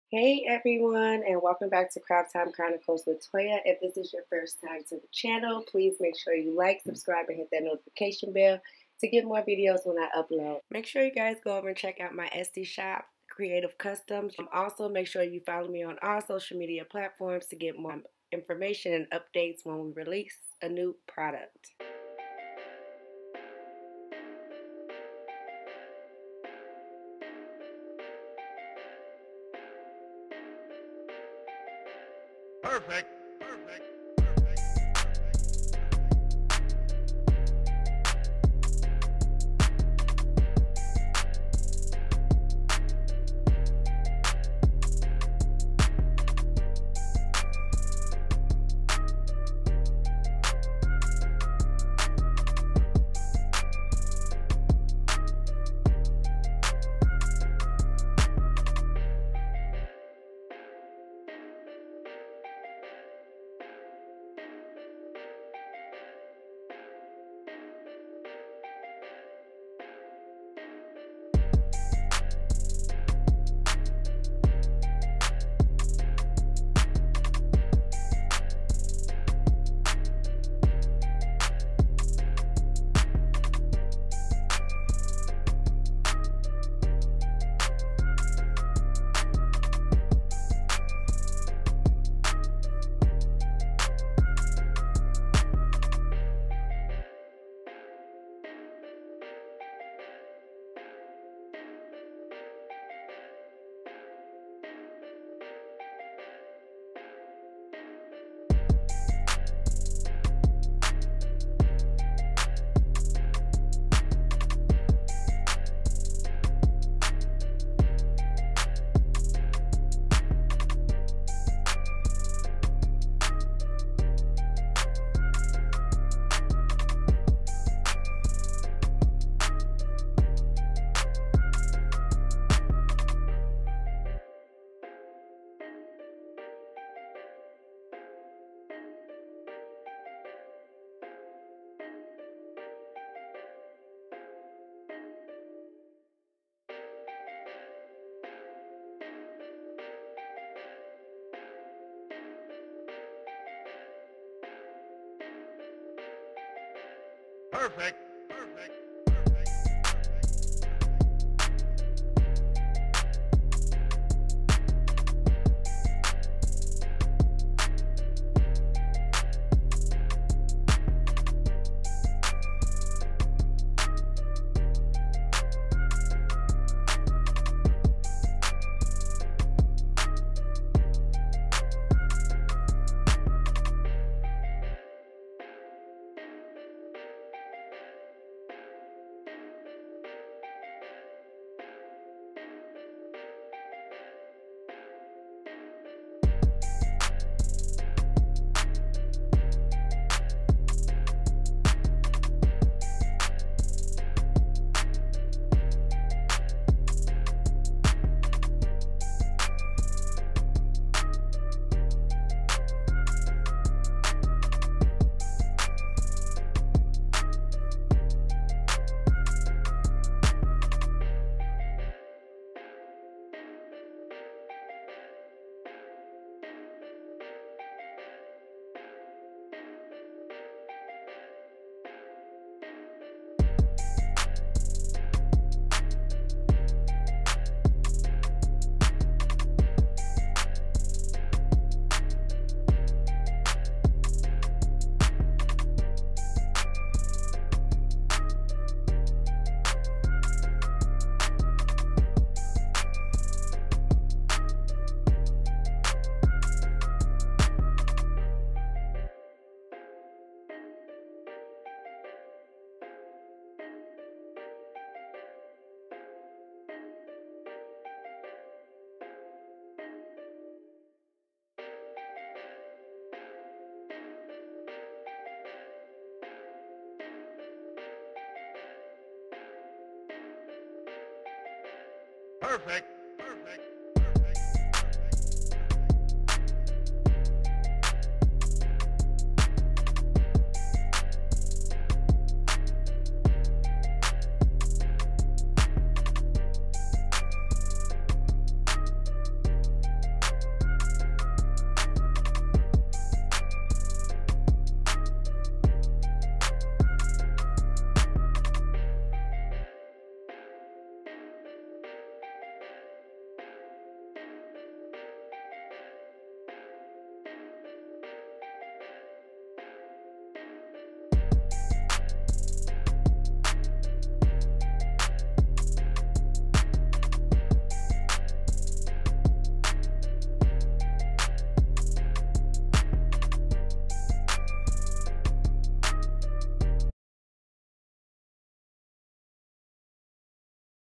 How to make personalized Tea Bag Holder | DIY | TUTORIAL | Made with Cricut
Easy DIY Customized Tea Bag Holder and Mug made with Vinyl.  Made with Cricut. Super Easy!